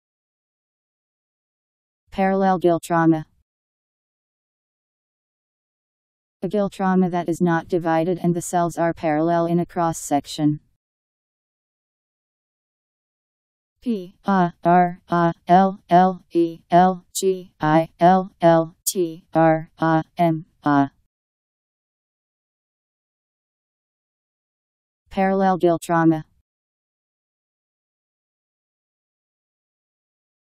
Parallel gill trama Meaning
Video shows what parallel gill trama means. A gill trama that is not divided and the cells are parallel in a cross section..  Parallel gill trama Meaning. How to pronounce, definition audio dictionary. How to say parallel gill trama. Powered by MaryTTS, Wiktionary